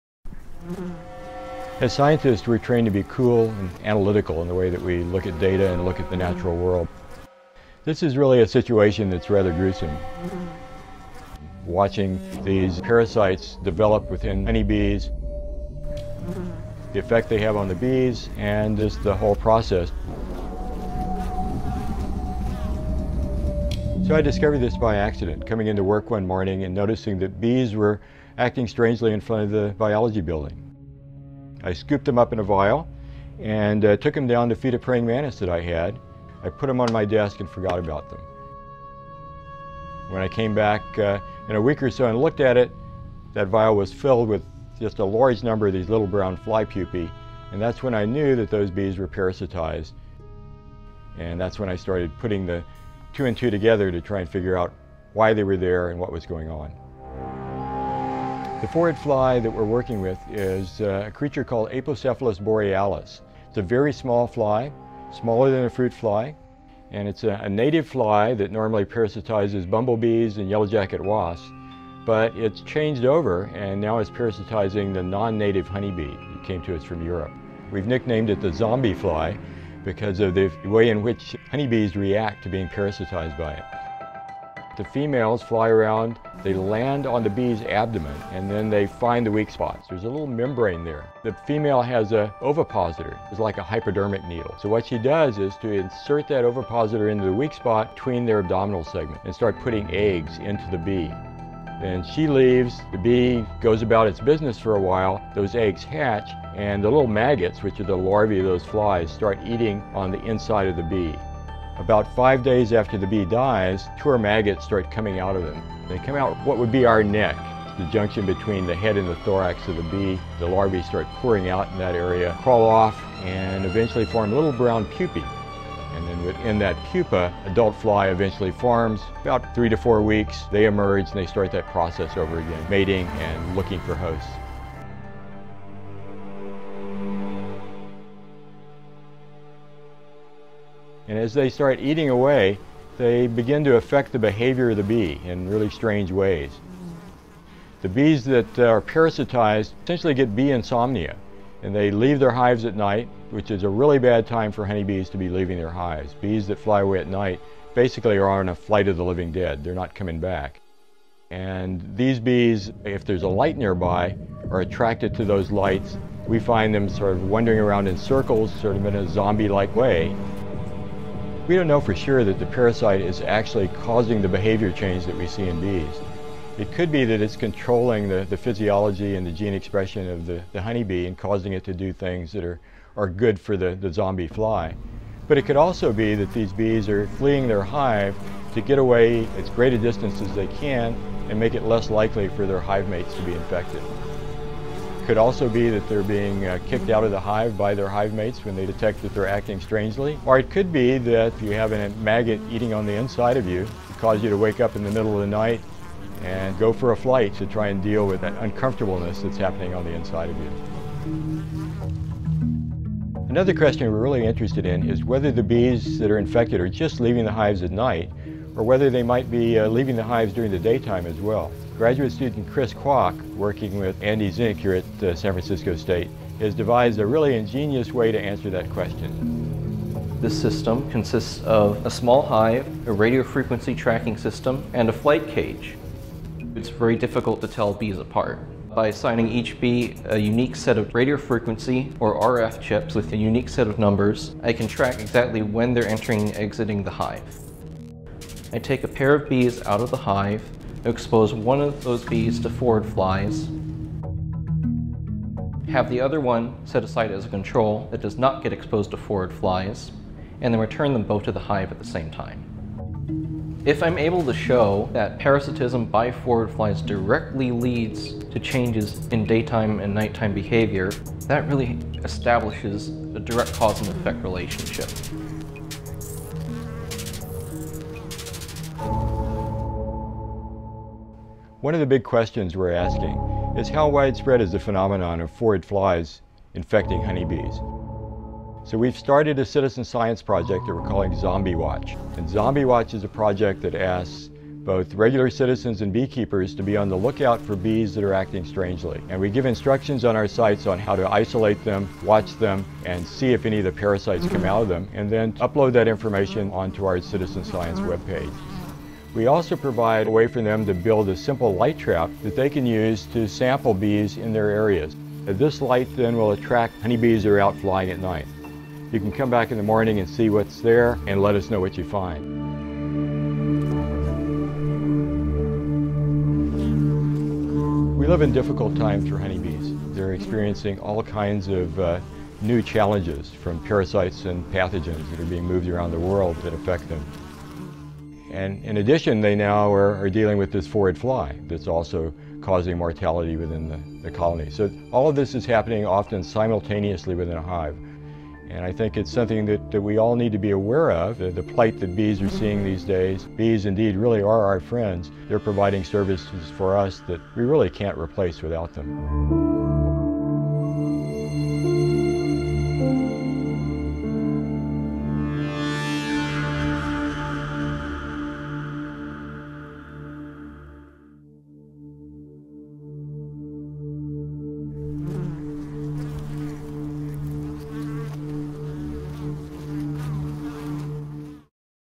ZomBees: Flight of the Living Dead
Something strange and unsettling is happening to Bay Area honeybees. Entomologists at San Francisco State University have identified the culprit: a tiny parasitic fly is causing the bees to exhibit bizarre nocturnal behaviors before suffering a gruesome demise.